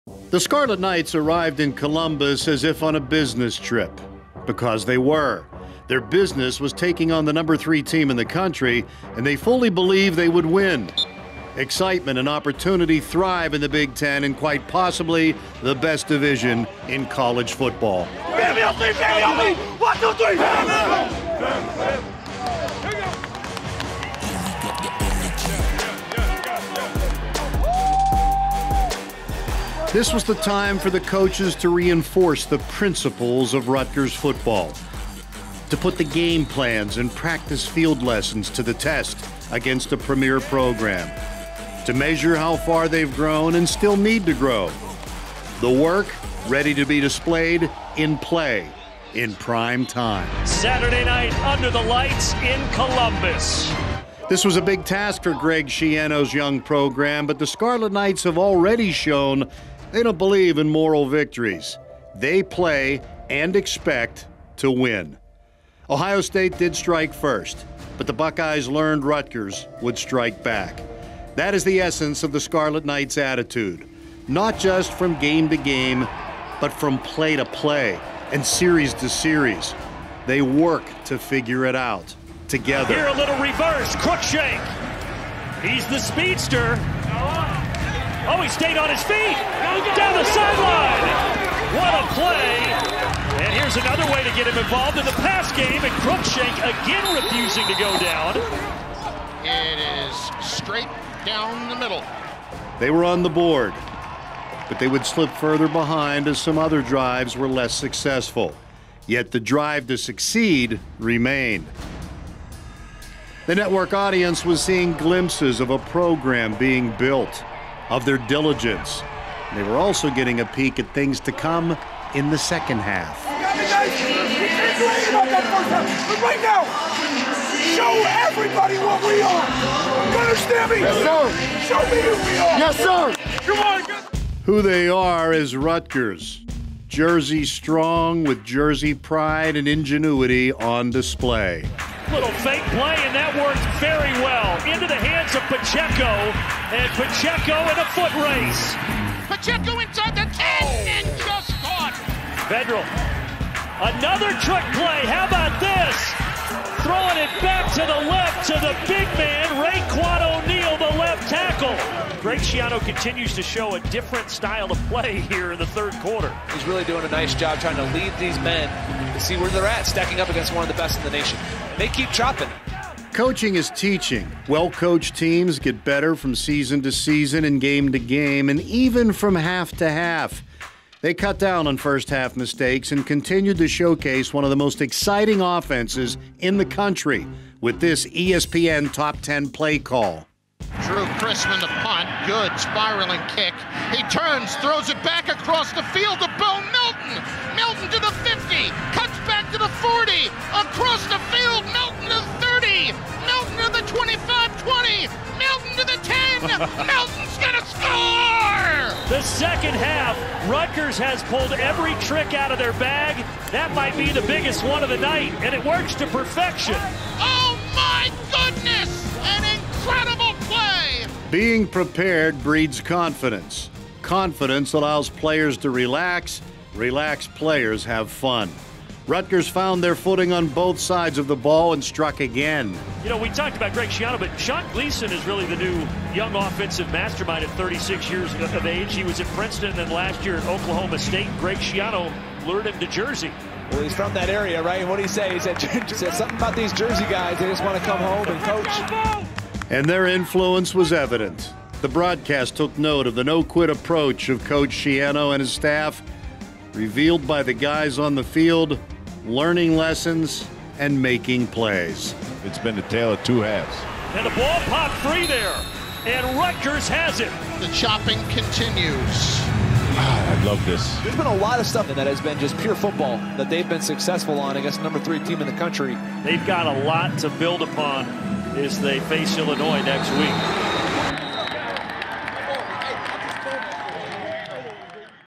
RFootball Game Story: Week 3 vs. Ohio State
The Scarlet Knights battled 'til the end in Columbus, showing signs of what's to come.